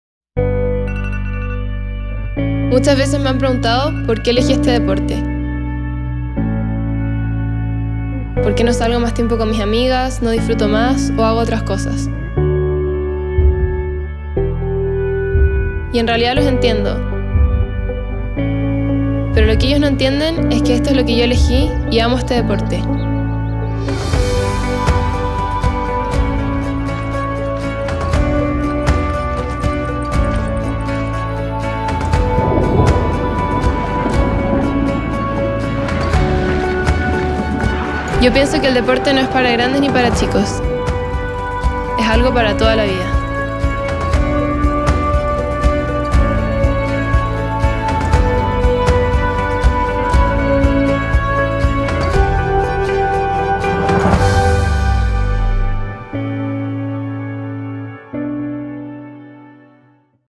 GATORADE® ⚡️ - IRONWOMAN - JOSEFINA FLORES
Ser diferente no es difícil cuando amas lo que haces. Josefina es una inspiración para todos aquellos que comienzan una carrera deportiva a corta edad y quieren seguir entregando su máximo esfuerzo y sudor por mucho tiempo más. Creemos en su espíritu, y muchos otras jóvenes que merecen llamarse IRONWOMAN. GATORADE®